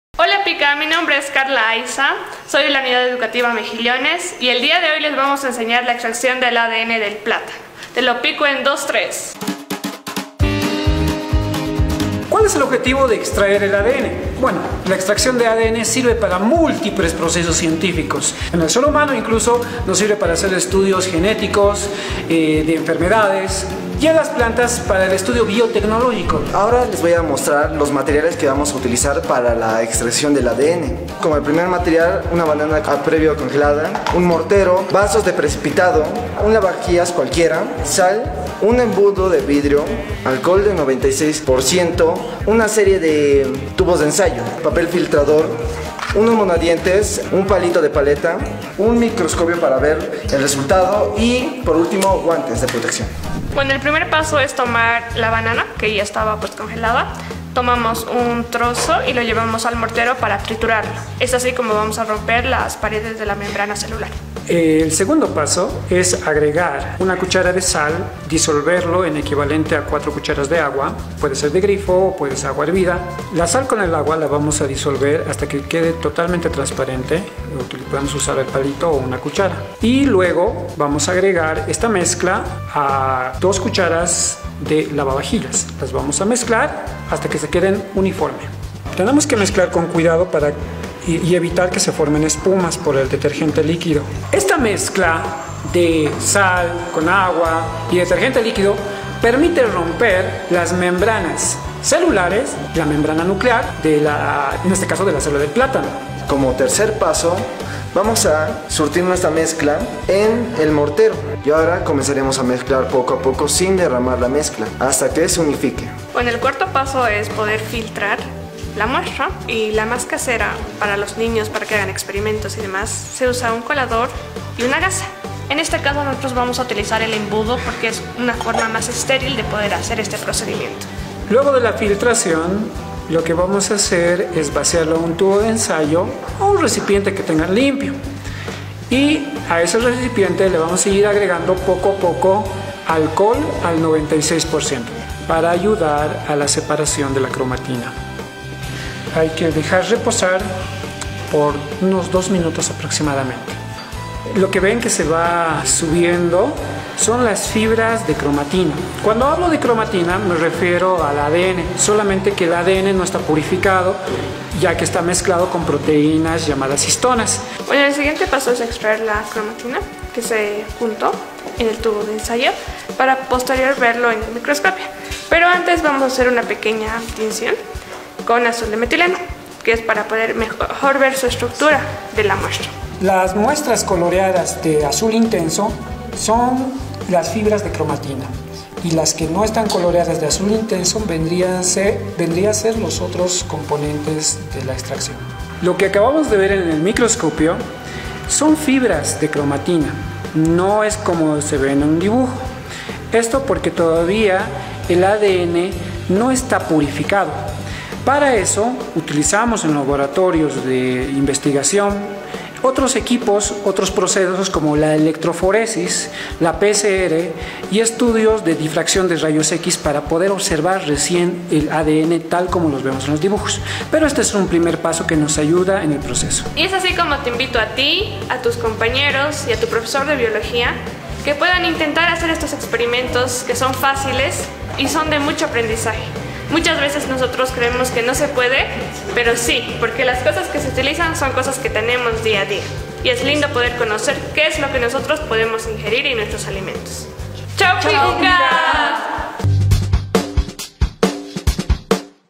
TE LO PICO EN 2,3 CÓMO EXTRAER EL ADN DE UN PLÁTANO
Junto a l@s estudiantes de la Unidad Educativa "Mejillones" La Paz 3 aprendimos a separar de una forma muy sencilla la cromatina del plátano.